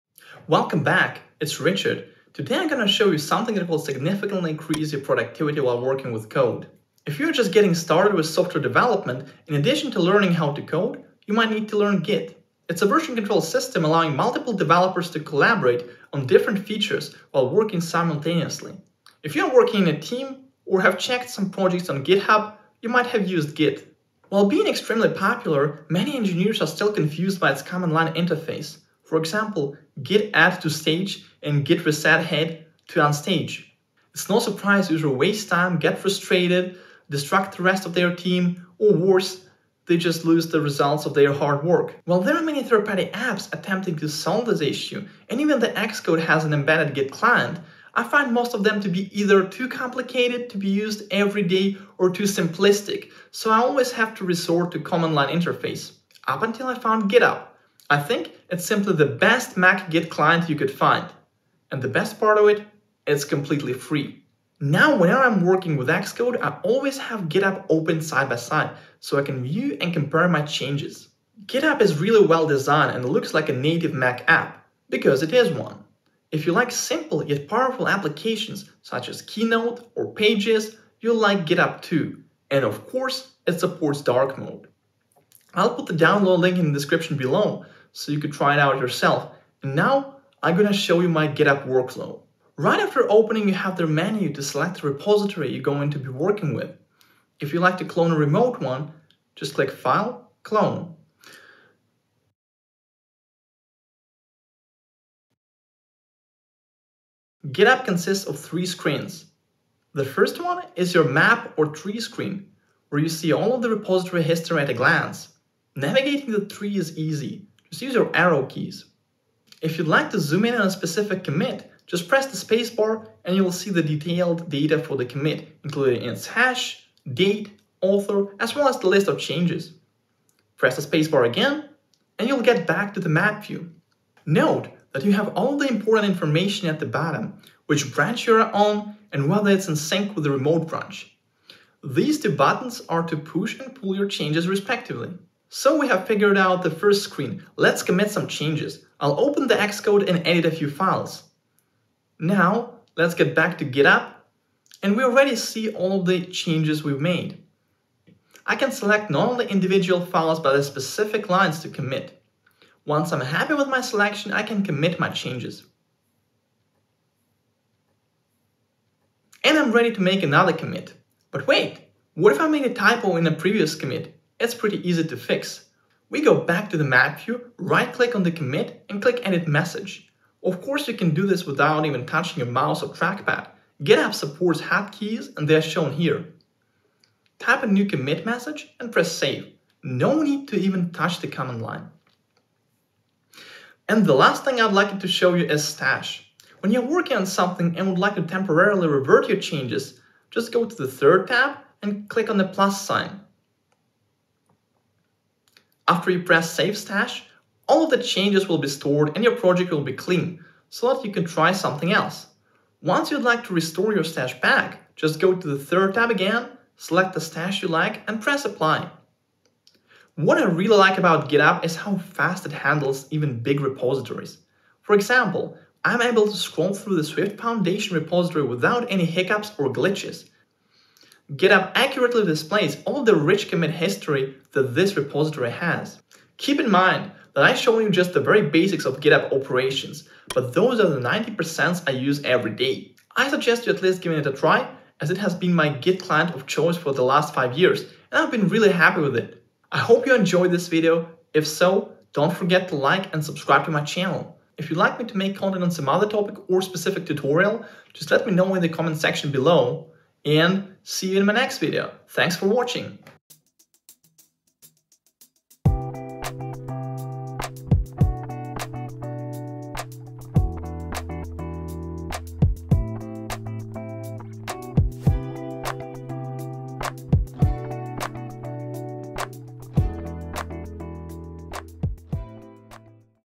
GitUp: the Best Git Client for Mac
Work quickly, safely, and without headaches. The Git interface you've been missing all your life has finally arrived.
I've been using GitUp as my main Git client for a few years and I consider it to be superior not only to free, but to all paid applications in it's category.
It's unique interactive repo graph, unlimited redo of almost all operations, and ridiculously fast UI make interacting with Git a pleasure.
GitUp is well suited for beginners but works as well for big projects with few tens of thousands of commits.

Timecodes:
0:00 Intro
0:24 Challenges of Git
0:56 GitUp
1:30 Cloning the repository
1:46 Interface overview
2:27 Committing the changes
2:50 Editing commits
3:12 Working with stash
3:40 Performance
3:56 Summary